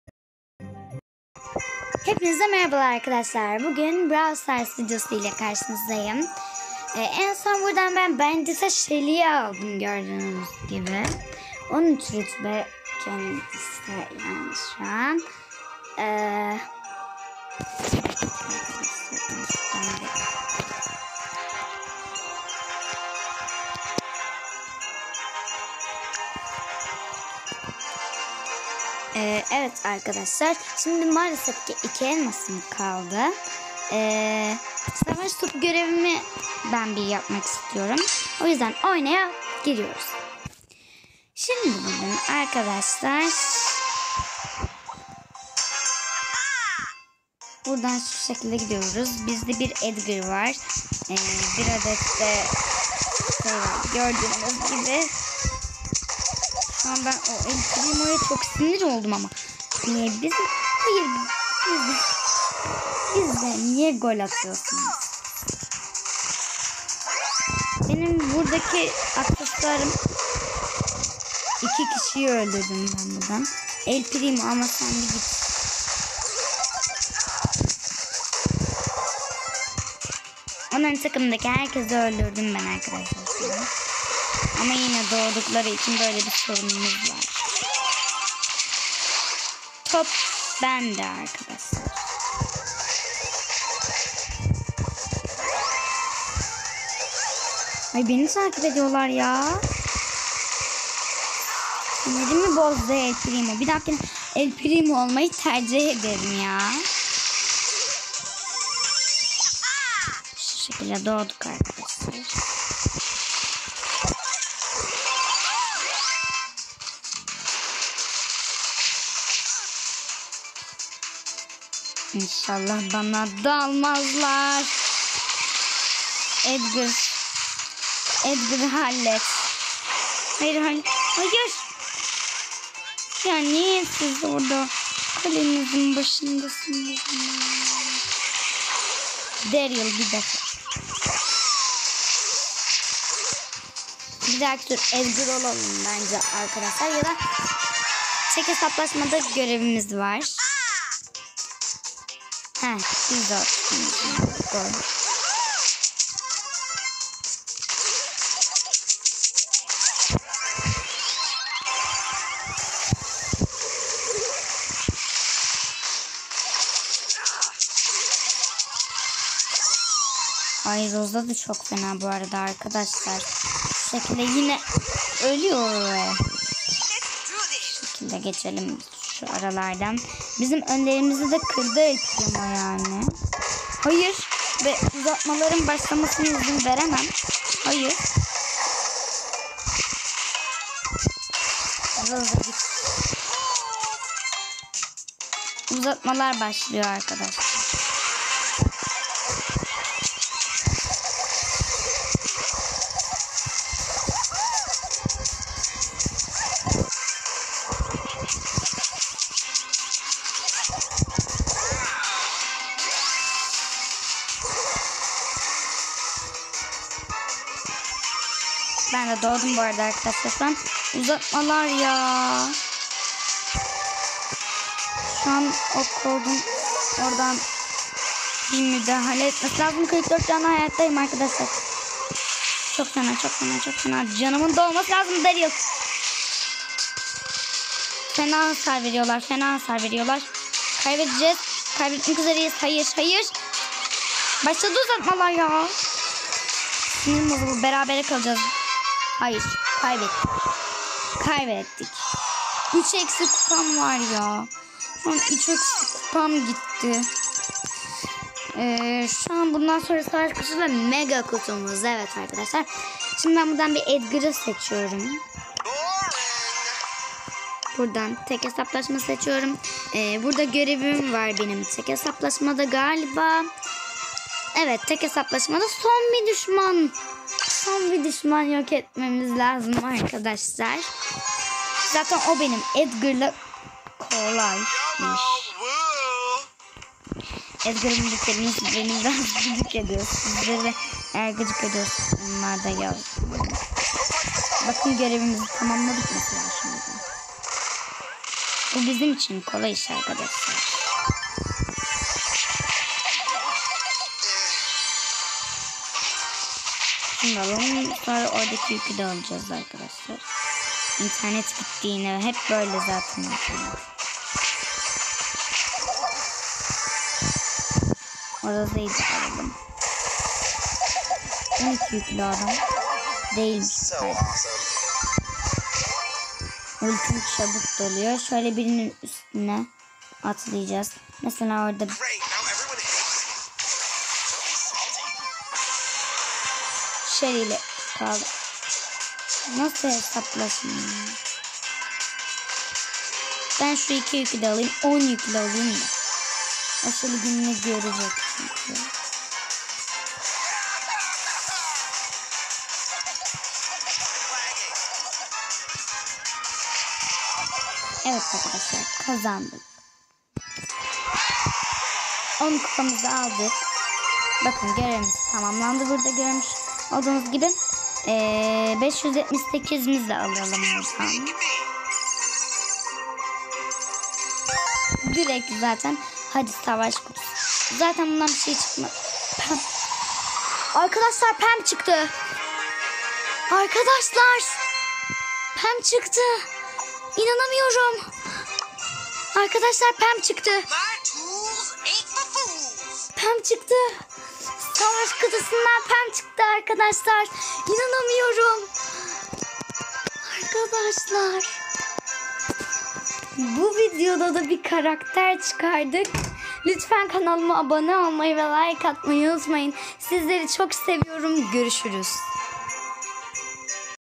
Taktik denedim savaş kutusundan Pam çıkardım! 😮😮
Taktiklerim devam edecek